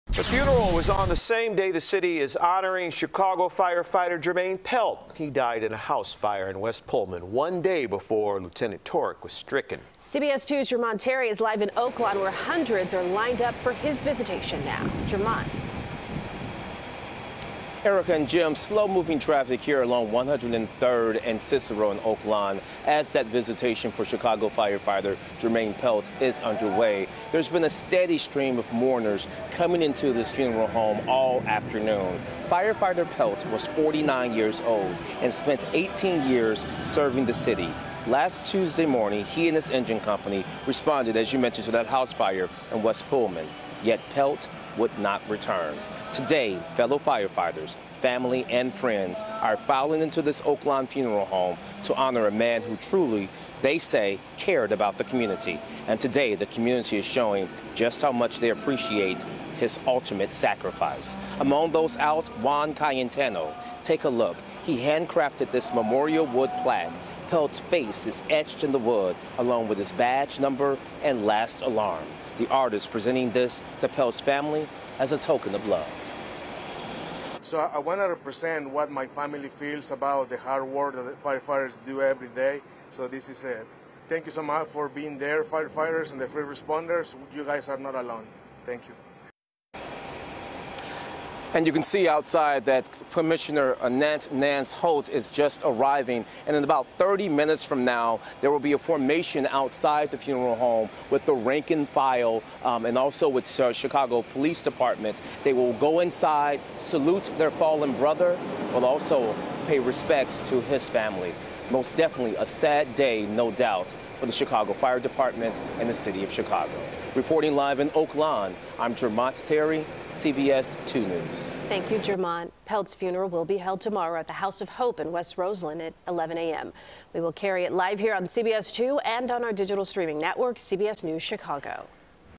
Mourners pay their respects to fallen CFD firefighter Jermaine Pelt
CBS 2's Jermont Terry reports from Oak Lawn, where hundreds lined up for his visitation on the same day as the funeral for fallen CFD firefighter Lt. Jan Tchoryk